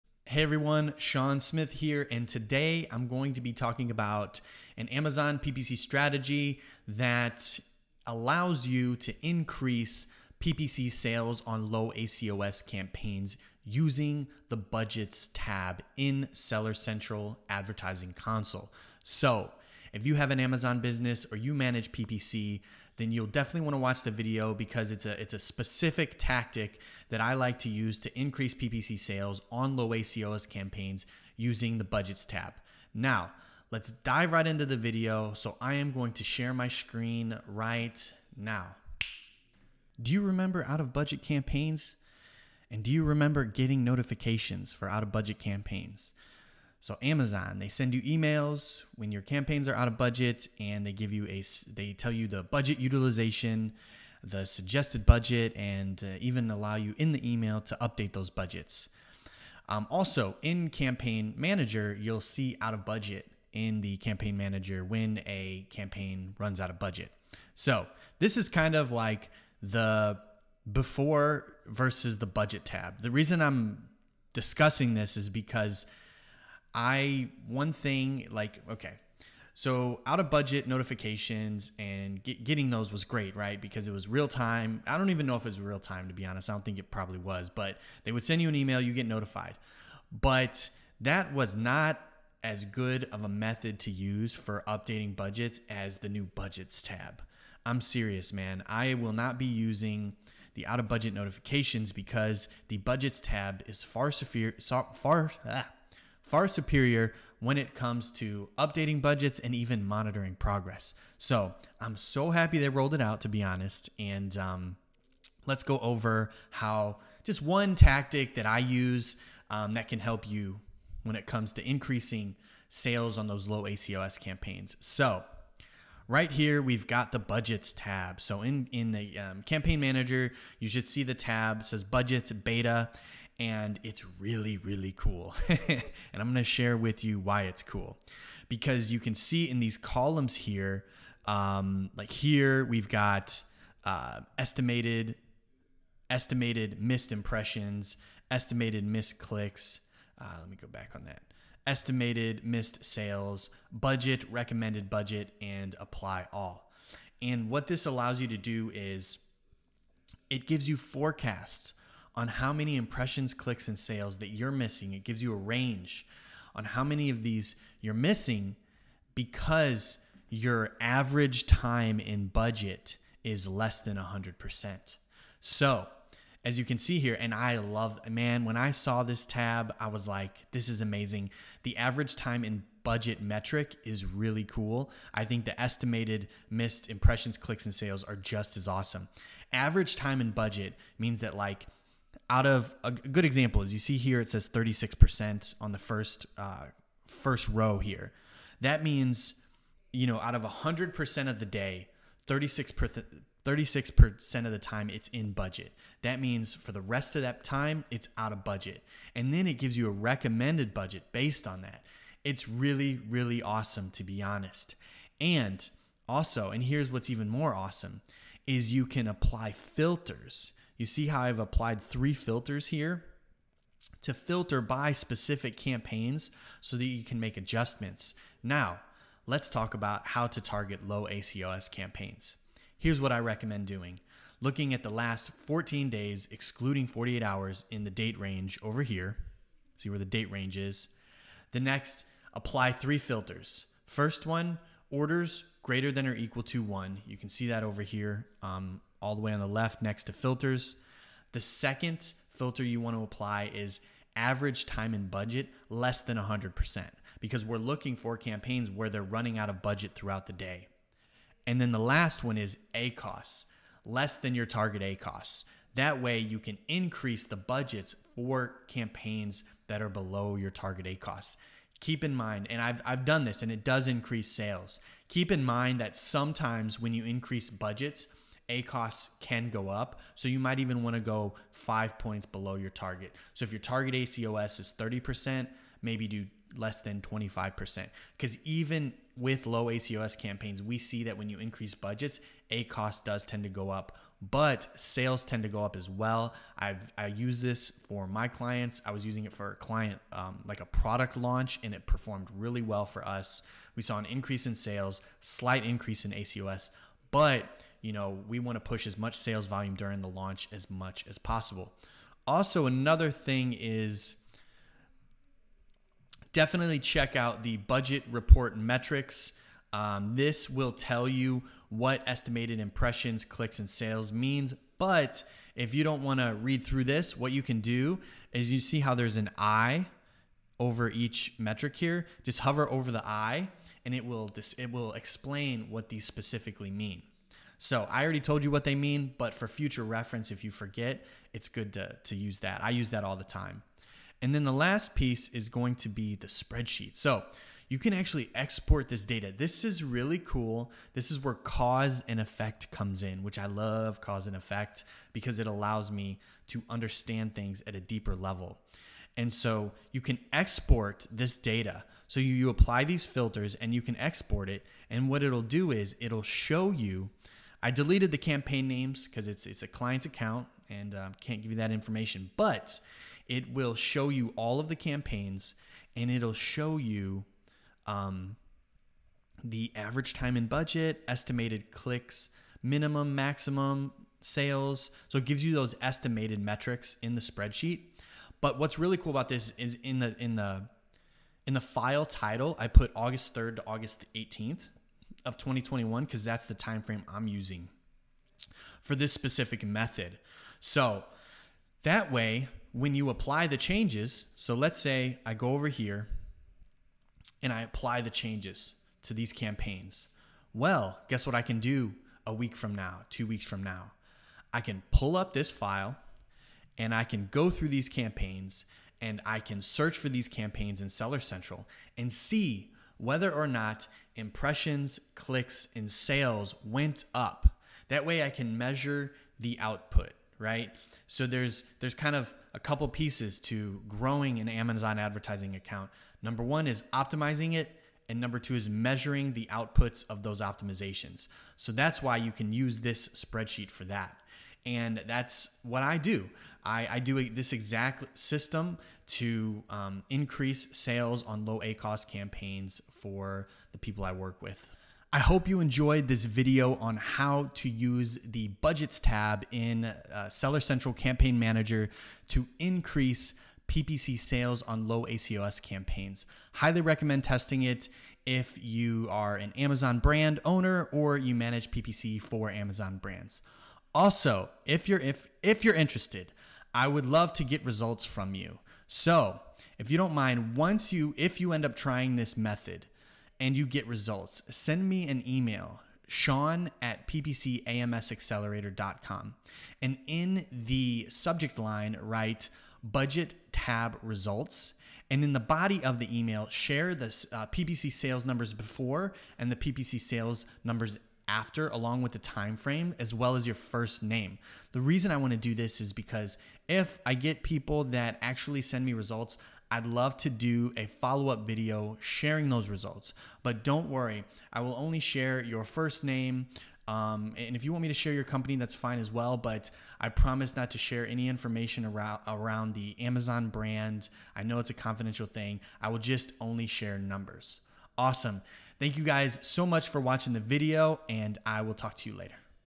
Amazon PPC Budget Strategy 2021 - Increase Sales on Low ACoS Campaigns with the Budgets Tab
Increasing PPC sales on low ACoS keywords and campaigns is considered the holy Grail of Amazon PPC.

In this video I cover an Amazon PPC strategy that I use to increase Amazon PPC budgets for campaigns with a lower ACoS.

I do this Amazon PPC optimization using the budgets tab.

This specific Amazon PPC tutorial can be used for Amazon sponsored products.

So if you are an Amazon private label seller or manage PPC for Amazon sellers this video is for you.

But before we cover how to maximize Amazon advertising budget lets discuss the way it used to be.

Before getting access to the budgets tab the only way to determine whether or not the campaign was out of budget was one of two ways.

You could manually log into your campaign manager and check to see if it says out of budget.

Or you could check to see if Amazon sent you an out of budget notification to your email.

But in my opinion these methods are not optimal.

So when they released the budgets tab in the Amazon ads console I decided to take time to learn more about it.

And what I learned blew my mind.

It showed me the average time in budget for each campaign.

It also showed estimated impressions, clicks, and sales I could get with those specific campaigns if I adjust the budgets.

Let me give you an example to show you what average time in budget means.

Let’s say that in the average time in budget column it shows 40%.

That means that the campaign is out of budget 60% of the time throughout the day.

Now that I have explained average time in budget lets dive right into the Amazon PPC tutorial.

For this specific Amazon strategy you are going to want to apply three filters when you are in the budgets tab.

The first filter is going to be orders greater than or equal to one.

The second filter is going to be average time in budget less than 100.

The third filter is going to be less than your target ACoS.

Doing this will show you all of the campaigns that are less than your target ACoS and are running out of budget throughout the day.

So, what do we do with this information?

Well, you are going to want to select all of the campaigns.

Then you are going to want to click the apply all button.

This will apply the recommended budgets to the campaigns.

This will increase budgets for campaigns below your target ACoS.

The goal here is to increase low ACoS amazon advertising sales.

The budgets tab is an awesome evolution in amazon ads.

It shows that Amazon is continuing to innovate its platform and give us tools to grow ppc sales.

Additionally, optimization tools like this give us the power to segment data and make better decisions.

Which is far superior to the advertising tools we had 5 years ago.

It’s like night and day.

I’ve been testing this amazon ads optimization with my clients and am seeing that it does increase sales.

I don’t have a ton of data for this specific optimization but the results I’m seeing are promising.

I forgot to mention that Amazon does a great job of explaining the definitions of terms when they roll out new features.

You can scroll your mouse next to the words to get details on what they mean.

You can even click to learn more about that PPC feature.

Additionally, campaign manager gives you the ability to export the data so you can store it for yourself.

I export the amazon ppc data so that I can refer to it after I make the changes.

Always remember that when it comes to Amazon advertising optimization our goal is to make changes, track them, and observe the results of those changes.

This allows us to develop a high level understanding of the platform.

And gives us insights so that we can make advanced ppc optimizations and get better results.

If you end up using this methodology and start seeing results I would love for you to send them to my email.

In the subject line write ‘Budgets tab Results’ and in the body of the email share the before and after as well as your first name.

I would love to do a follow up video to this sharing peoples results.

I promise not to share any information specific to your brand. Only numbers.

I will also email you to let you know when I make the video sharing results.